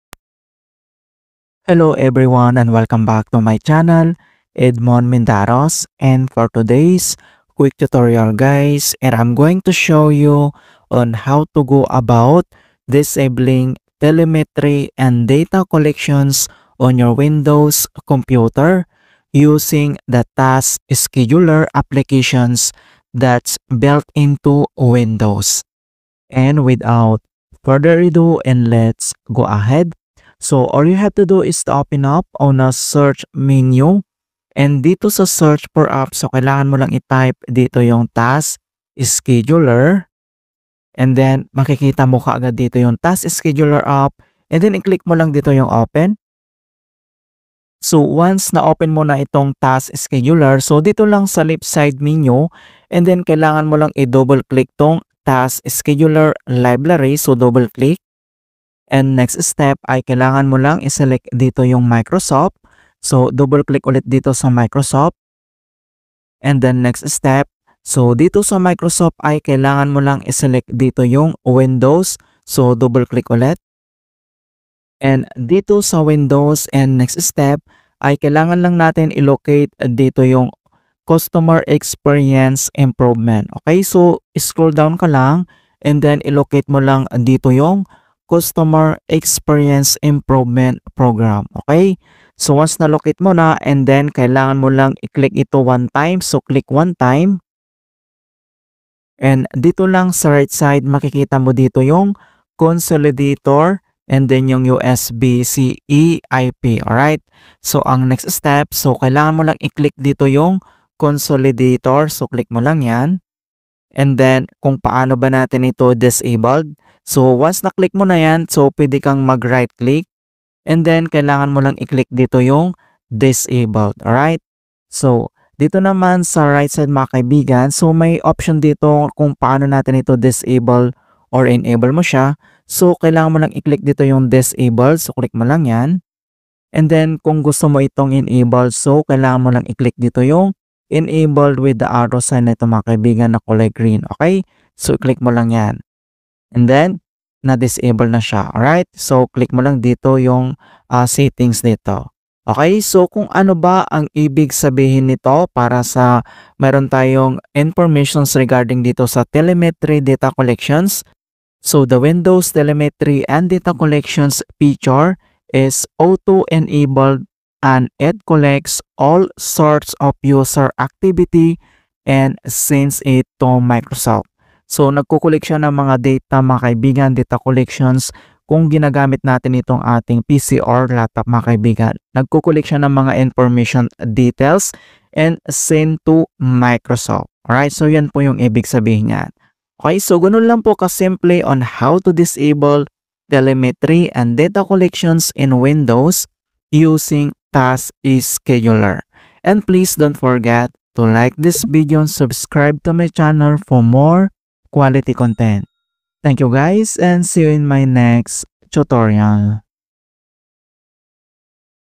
How To Disable Telemetry and Data Collection in Windows Using Task Scheduler
In this tutorial, we will show you how to disable Telemetry and Data Collection using Task Scheduler in Windows 11/10. The Windows Telemetry and Data Collection feature is auto-enabled and it collects all sorts of user activity and sends it to Microsoft. Windows telemetry is vital technical data from Windows devices about the device and how Windows and related software are performing.

Microsoft collects data through its telemetry system that was introduced with Windows 10. Microsoft collects the data to improve your user experience and monitors your Windows settings, the apps you use, and system settings, among other things.

Disable Telemetry and Data Collection using Task Scheduler:
1. Press Windows key + R to invoke the Run dialog.
2. In the Run dialog, type taskschd.msc.
3. Press CTRL+SHIFT+ENTER key combo to open Task Scheduler in admin mode.
4. In the window that opens, on the left pane, navigate to the path below: Task Scheduler Library / Microsoft / Windows / Customer Experience Improvement Program.
5. At the location, on the middle pane, right-click on the task named Consolidator and select Disable from the context menu.
6. Repeat the process with all other tasks listed in the middle pane.
7. Exit Task Scheduler when you’re done.

Disable Telemetry Using the Group Policy Editor:
1. To launch the Group Policy Editor, press Win + R, type gpedit.msc, and press Enter. Use the left sidebar to navigate to Computer Configuration / Administrative Templates / Windows Components / Data Collection and Preview Builds.
2. Switch to the right pane and double-click Allow Diagnostic Data.
3. Once you double-click, a window should pop up. By default, it's set to Not Configured. Select Disabled instead and click OK.